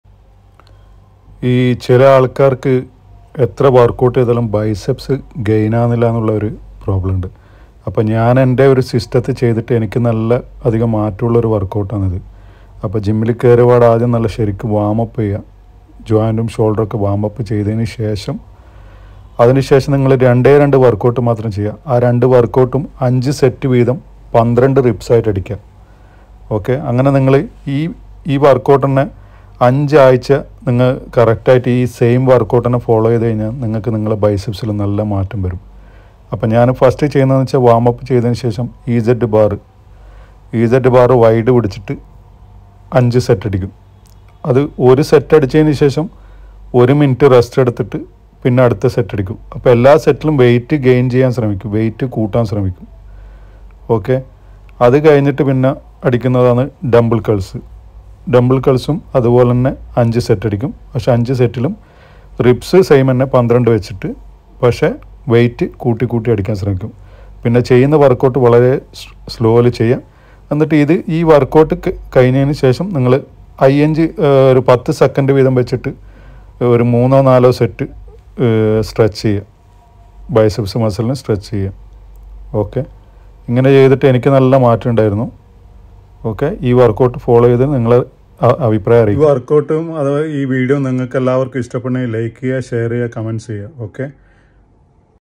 നിങ്ങൾക്ക് ബൈസെപ് മസിൽ വലുതാവണമെങ്കിൽ ഇത് പോലെ ചെയ്യുക
Biceps gain workout
My style
How to gain biceps muscle
Do this workout weekly 2 time
Only this two workout
12/5 sets